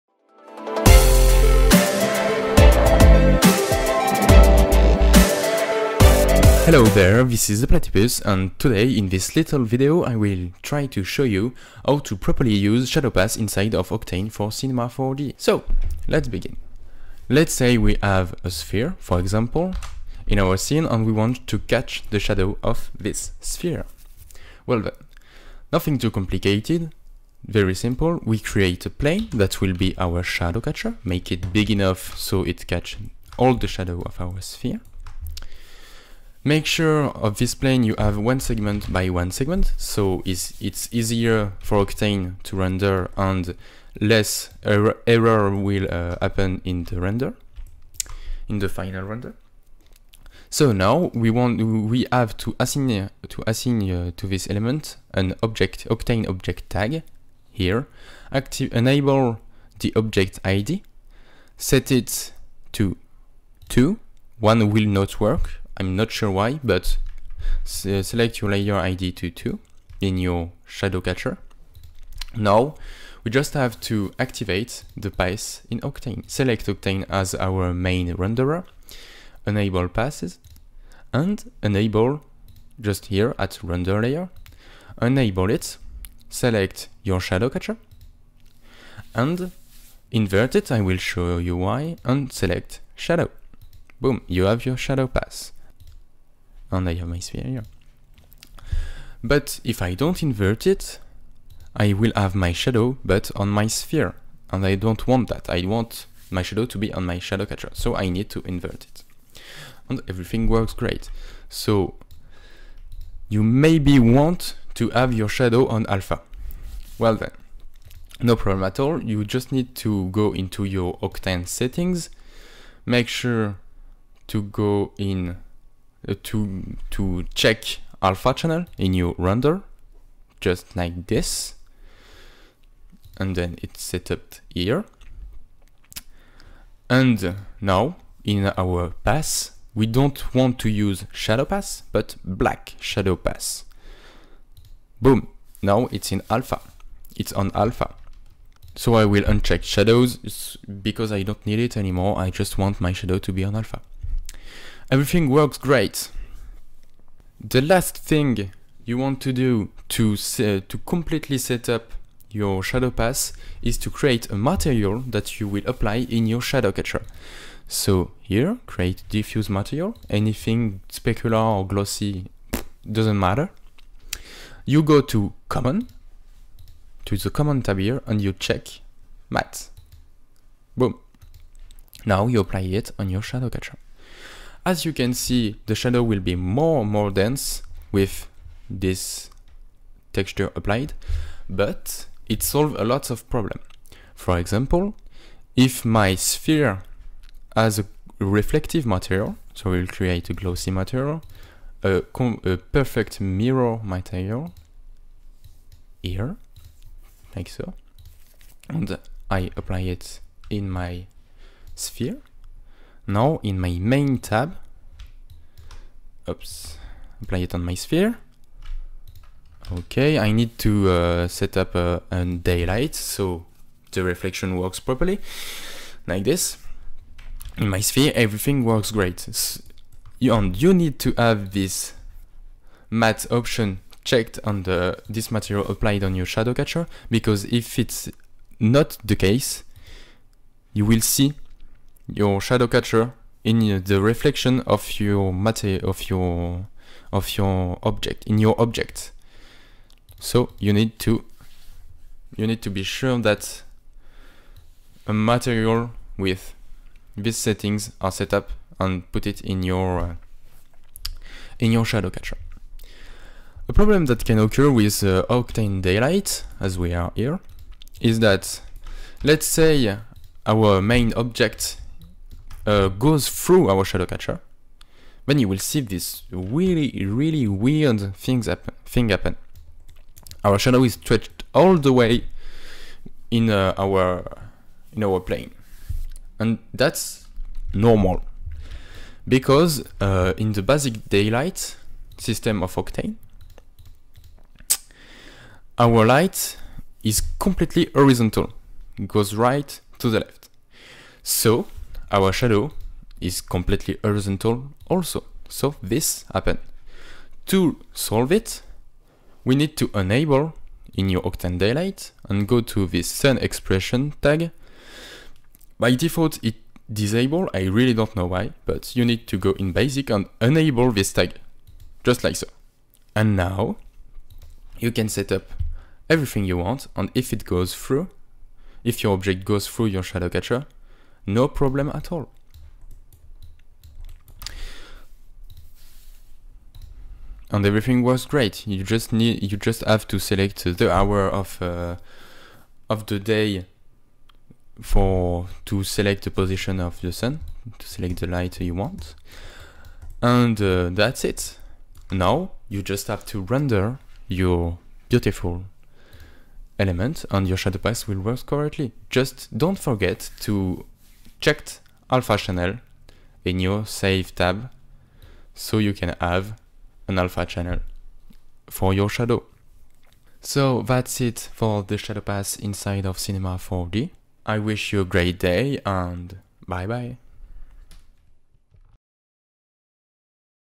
TUTO Octane c4d how to properly use shadow pass (Fix problems and errors)
Hello,

Hope my english isn't so bad, at least understandable. I encountered many problems with this Octane pass so that's why I made a quick video to recap all of it for those who are strugling with this shadow pass.

Other videos like this exist but If I remember right they don't always show it all.